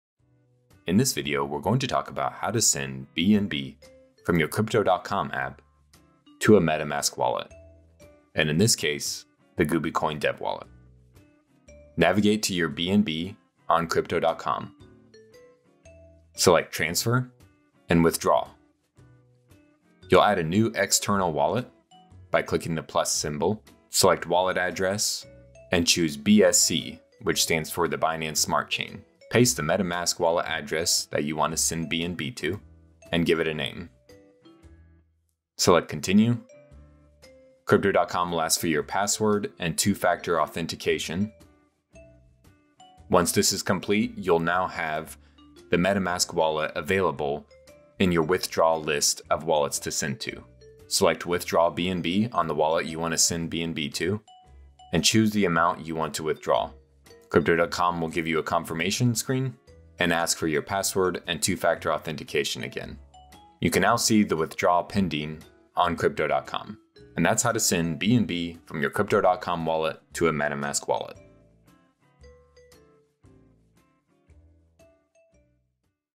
How to Send BNB from Crypto.com to a Metamask Wallet
In this video we walk through the basic steps of sending BNB from a crypto.com wallet to metamask wallet.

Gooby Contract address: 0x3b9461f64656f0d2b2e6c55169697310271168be